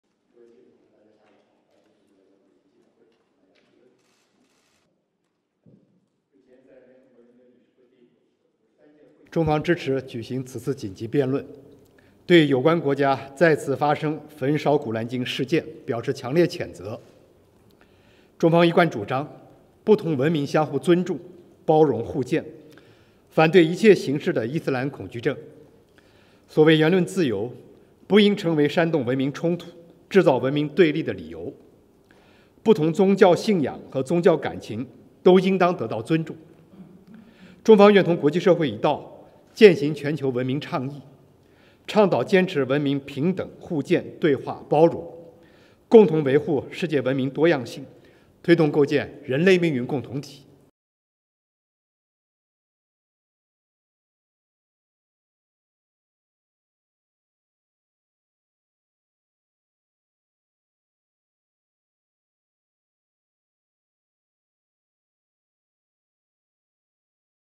China Condemns Religious Animosity of Burning Quran: FM Spokesman
China strongly condemns the religious animosity of burning the Quran and the discord-sowing provocation, and calls on the global community to jointly safeguard the diversity of world civilizations, said Chinese Foreign Ministry spokesman Wang Wenbin in Beijing on Thursday.